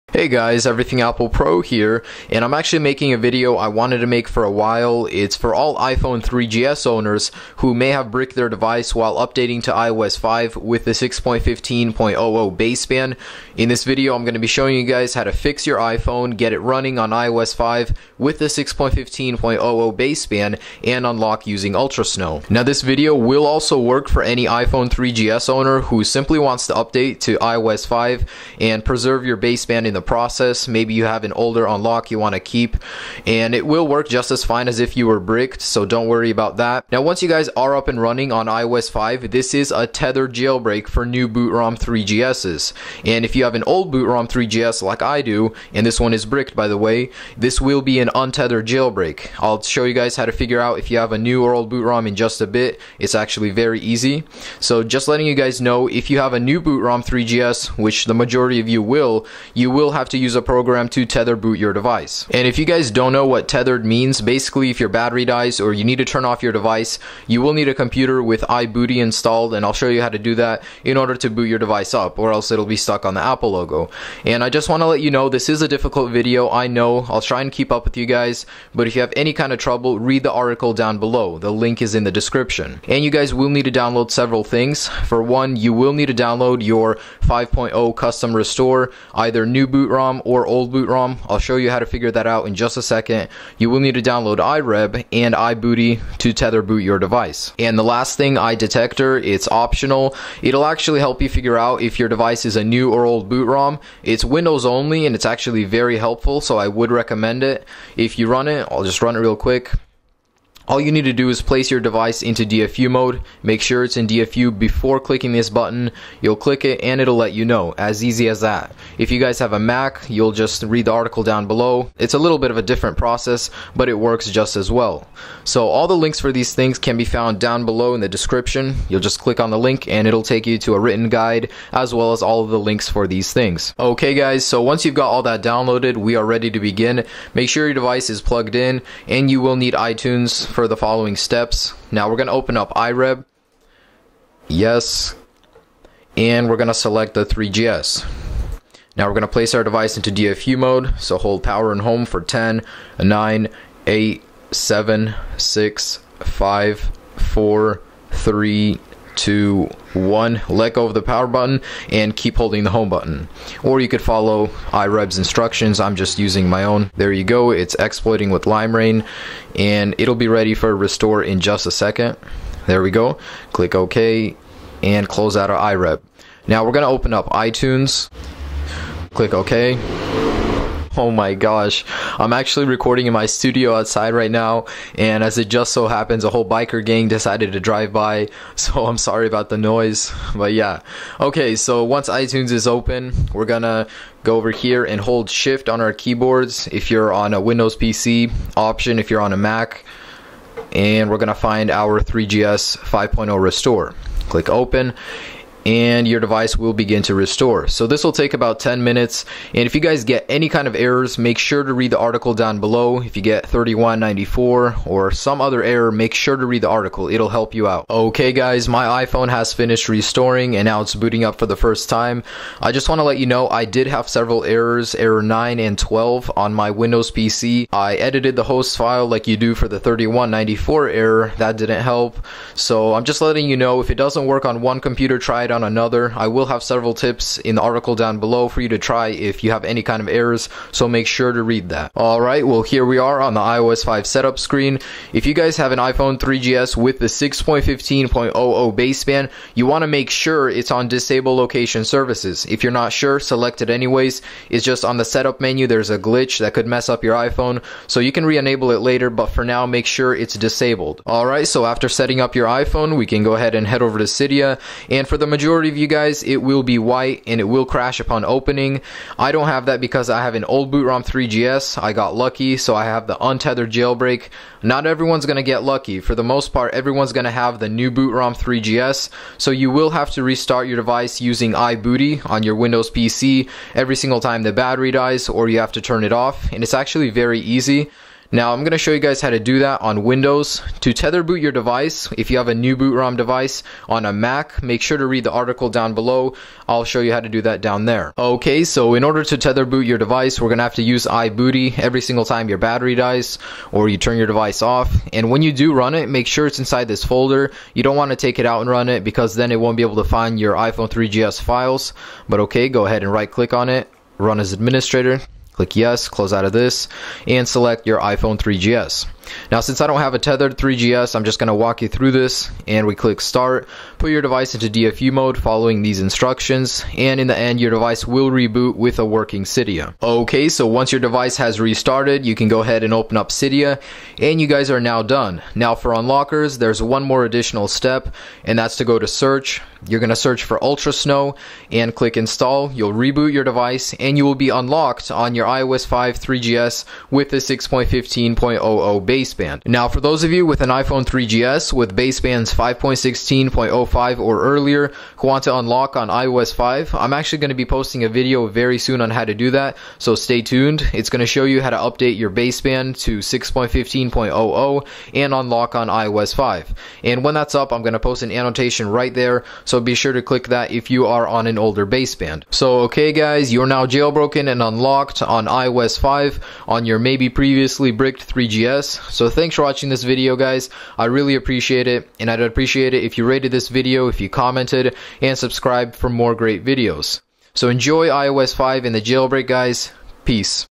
Restore & Update a Bricked iPhone 3Gs 6.15.00 to iOS 5.0 Firmware - 5.0 Jailbreak/Unlock
Restore & Update a Bricked 3Gs to iOS 5.0 on 6.15.00 Baseband & Unlock! Untethered Jailbreak for Old bootrom 3Gs. Will work for people simply looking to update their 3Gs to 5.0 and Preserve the Unlocked Baseband.